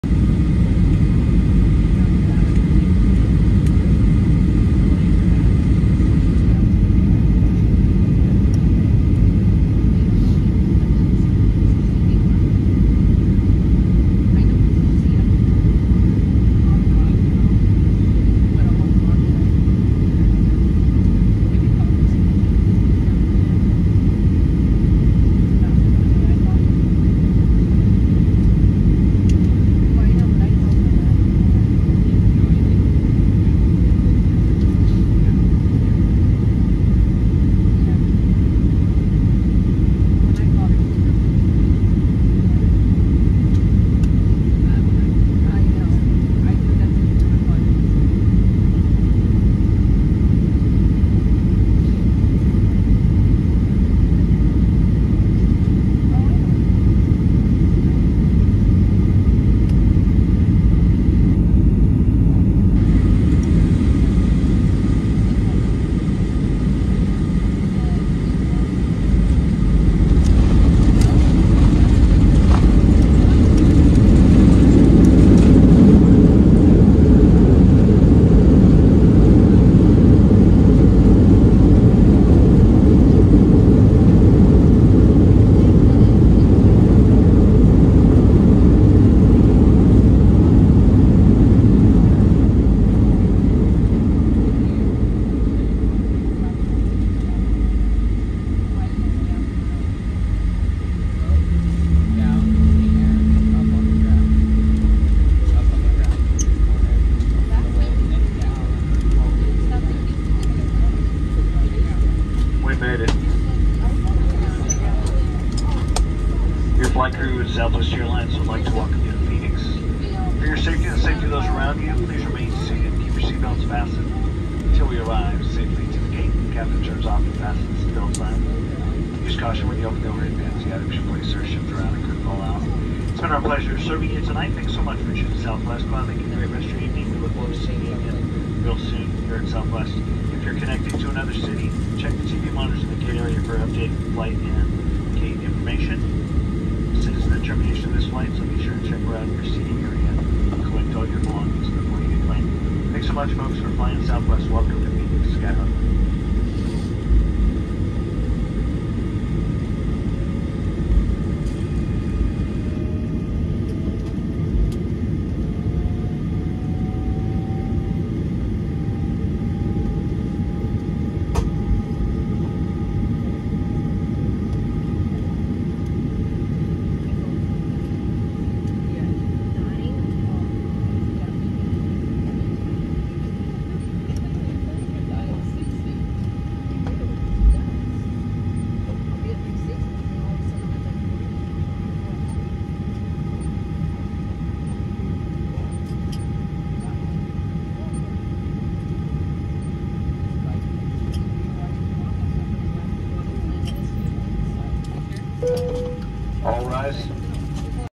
Southwest Airlines Boeing 737-800 Landing in Phoenix Sky Harbor International Airport (PHX)
Departed from: Houston-Hobby Airport